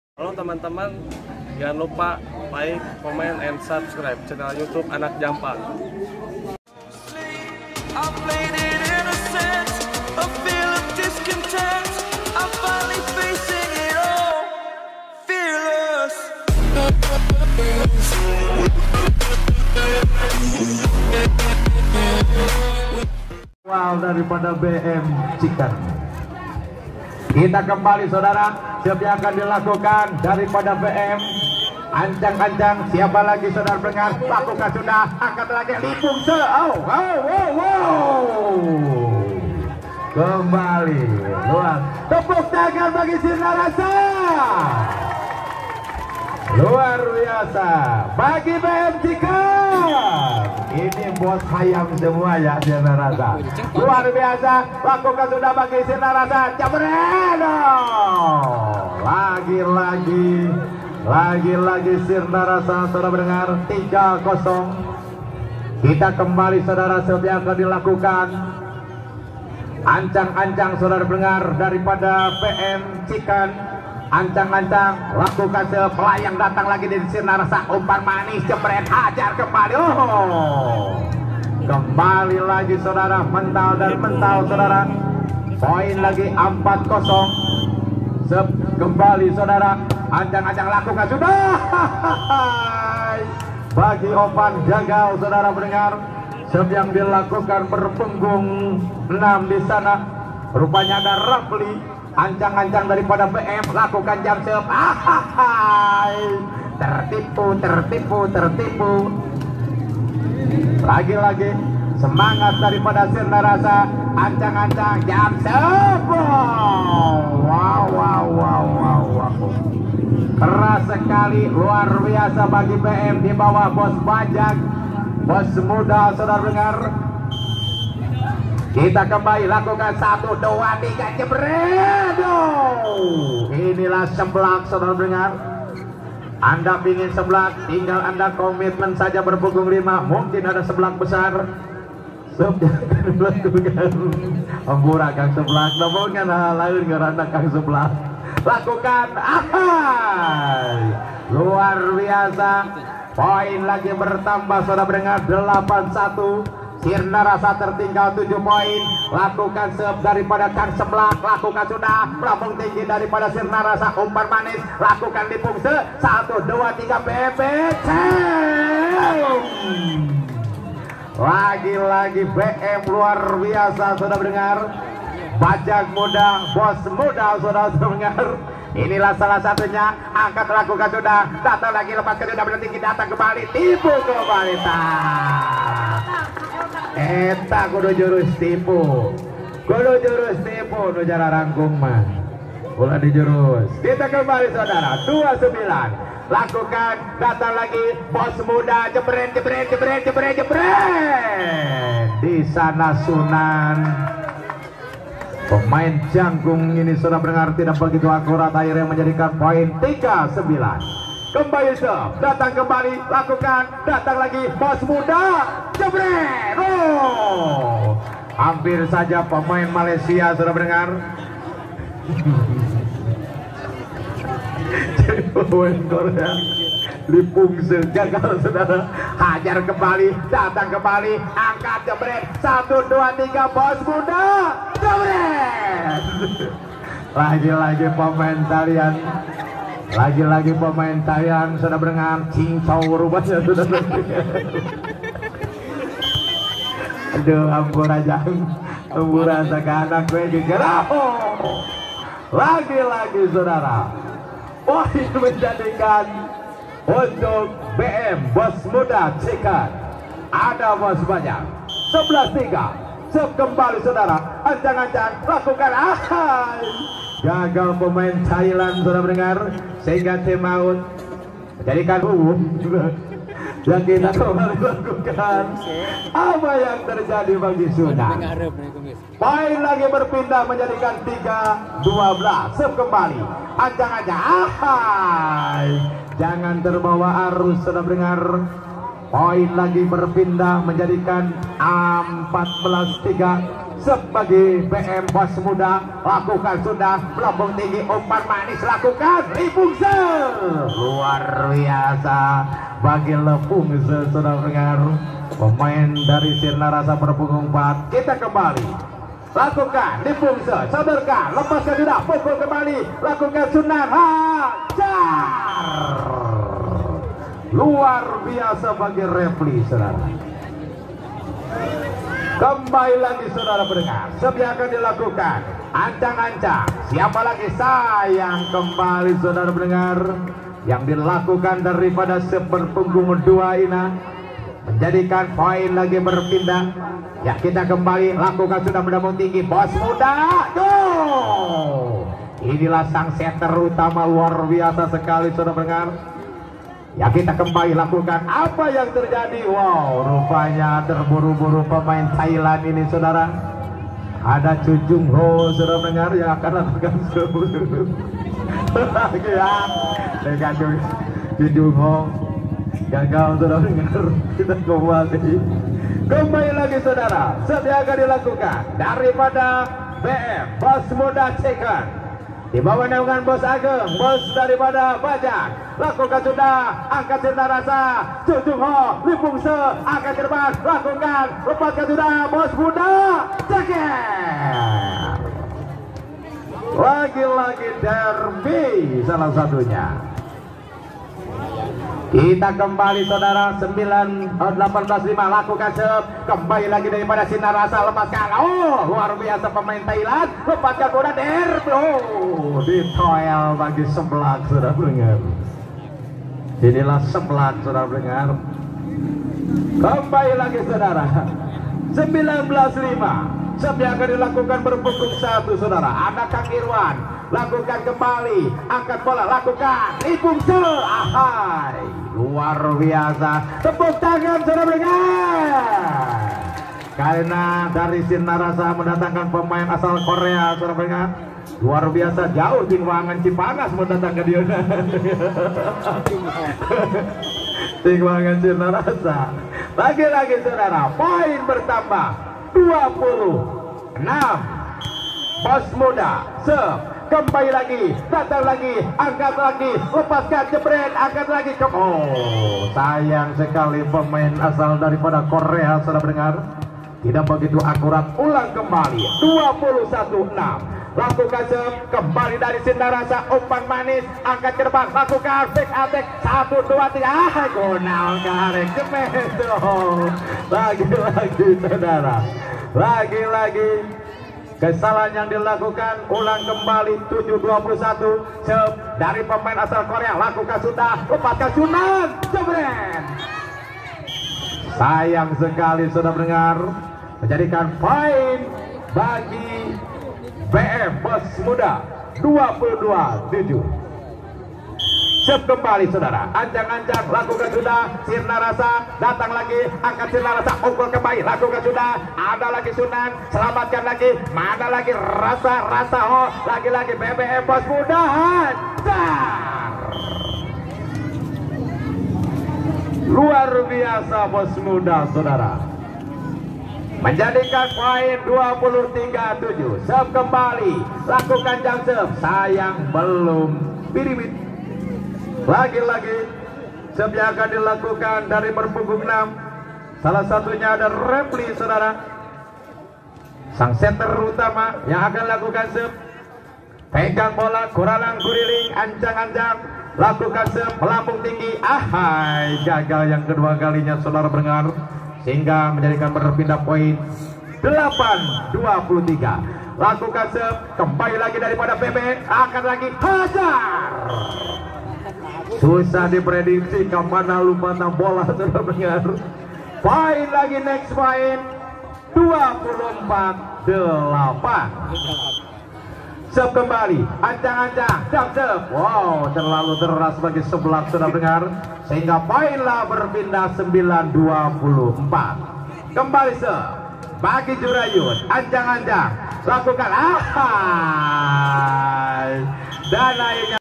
3TRIO PERUSUH SUNAN DERBI & RAFLY DI DATANGKAN UNTUK HADAPI KING TARKAM SUKABUMI !! ANAK JAMPANG
3trio perusuh sunan derbi & rafli di datangkan untuk hadapi king raja tarkam sukabumi !! anak jampang